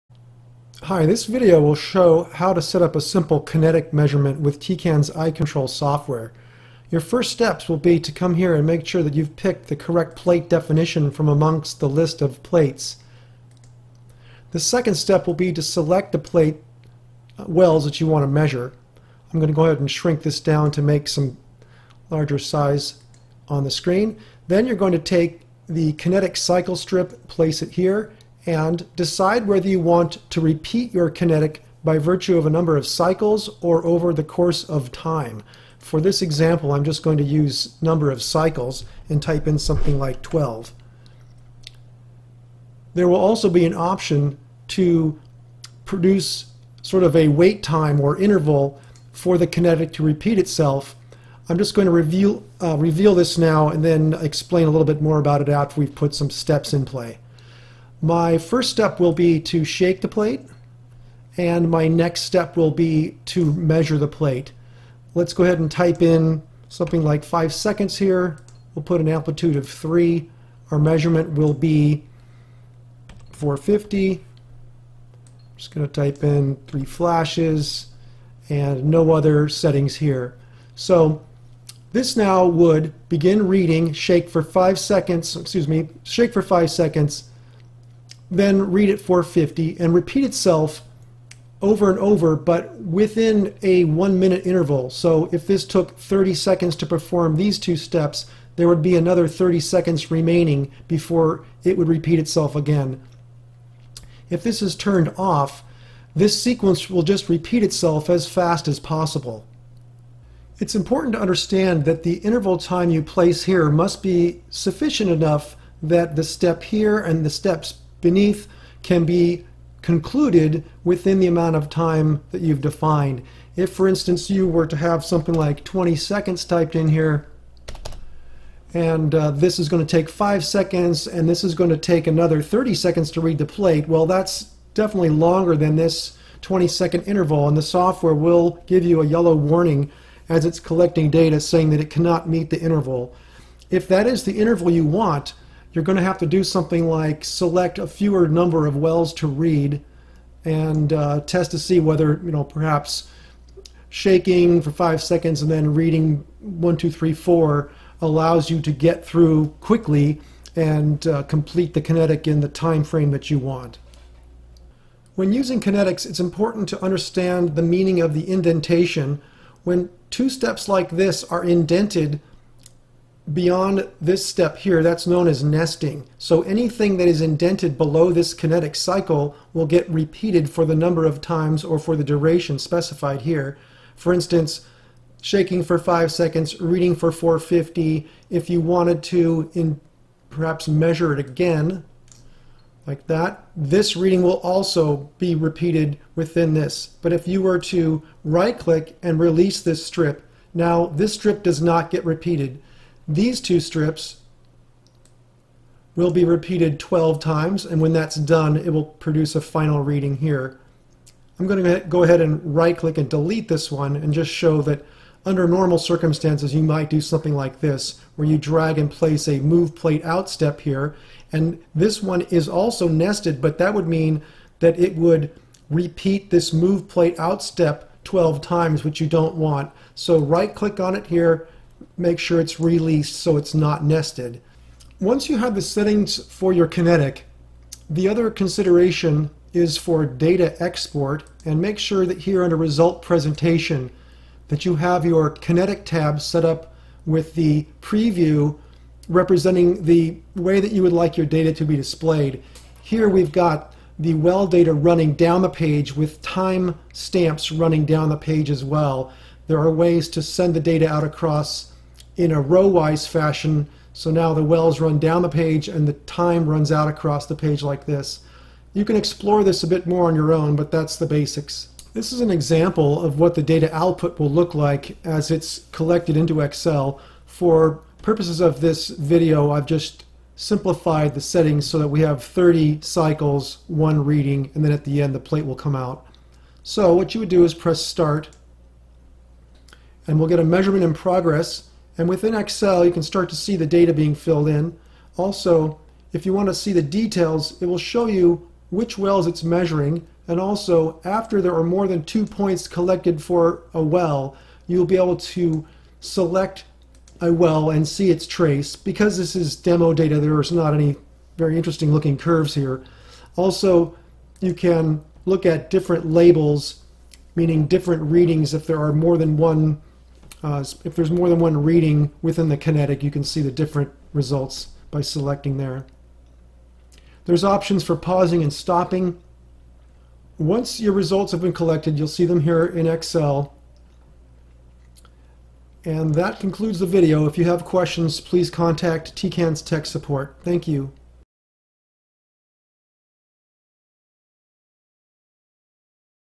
Tecan's iControl Software - Kinetic Measurement Basics part 1
This video explains how to set up a simple kinetic reading in iControl software.  This example is relevant to repeat measurements of the entire plate (aka plate-wise readings).  For well-wise kinetics where kinetic events are measured and completed in one well before moving on to another well, look for a Part 2 video on this channel for Well Wise Kinetics.